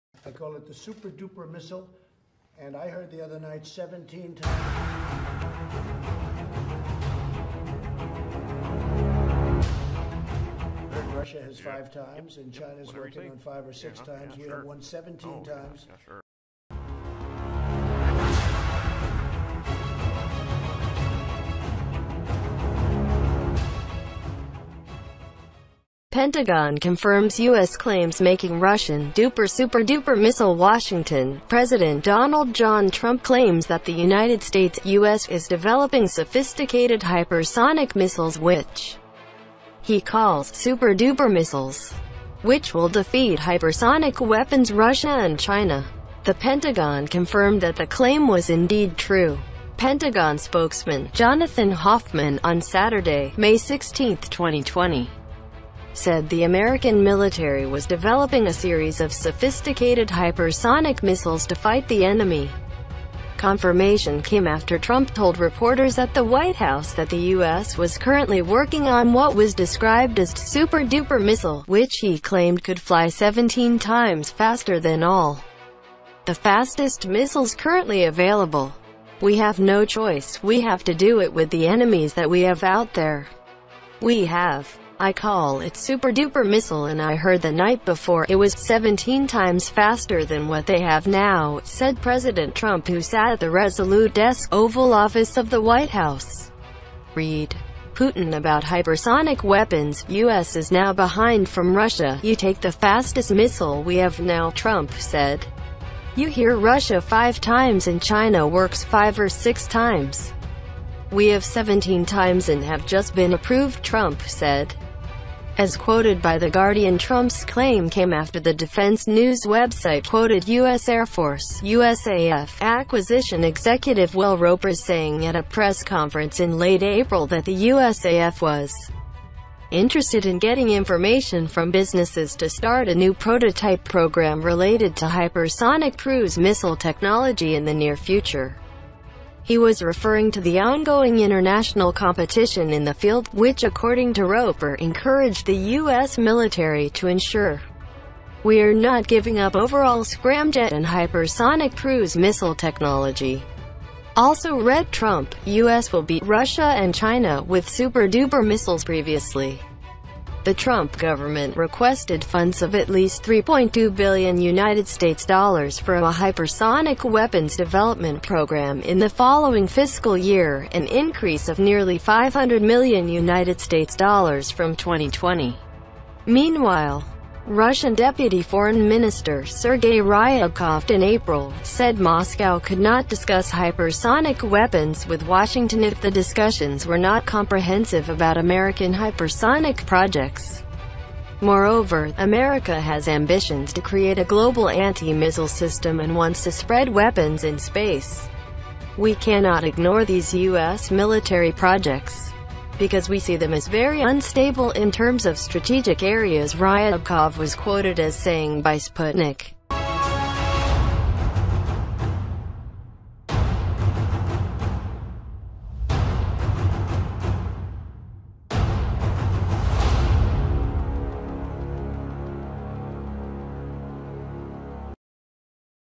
Pentagon Confirms US Claim to Make Russian Duper Super Duper Missile
WASHINGTON - President Donald John Trump claims that the United States (US) is developing sophisticated hypersonic missiles which he calls "super duper missiles", which will defeat Russian and Chinese hypersonic weapons. The Pentagon confirmed that the claim was indeed true.

Pentagon spokesman, Jonathan Hoffman, Saturday (05/16/2020), said the American military was developing a series of sophisticated hypersonic missiles to fight the enemy.

Confirmation came after Trump told reporters at the White House that the US was currently working on what he called a "super duper missile" which he claimed could
firing 17 times faster than all the fastest missiles that exist today.

We have no choice, we have to do it with the enemies that we have out there. "We have, I call them super duper missiles and I heard the night before (it was) 17 times faster than they have now," said President Trump who sat at the Resolute Desk, the Oval Office of the White House. (Read: Putin on Hypersonic Weapons: The US Is Now Behind from Russia)

"You took the fastest missile we have now," Trump said. "You hear Russia five times and China works five or six times, we have 17 times and have just been approved," Trump said, as quoted by The Guardian.

Trump's claim came after the Defense News website quoted US Air Force (USAF) acquisition executive Will Roper as saying at a press conference in late April that the USAF was interested in getting information from businesses to start a new prototype program related to hypersonic cruise missile technology. in the near future.

He was referring to the ongoing international competition on the ground, which according to Roper encouraged the US military to ensure; "We are not giving up scramjet technology and overall hypersonic cruise missiles." US Will Beat Russia and China with Super Duper Missiles)

Previously, the Trump government requested funds of at least USD3.2 billion for a hypersonic weapons development program in the following fiscal year, an increase of nearly USD500 million from 2020.

Meanwhile, Russian Deputy Foreign Minister Sergey Ryabkov, in April, said Moscow could not discuss hypersonic weapons with Washington if the discussion was not comprehensive about American hypersonic projects. In addition, America has ambitions to create a global anti-missile system and wants to spread weapons in space.

"We cannot ignore these US military projects, because we see them as very unstable in terms of strategic areas," Ryabkov was quoted as saying by Sputnik.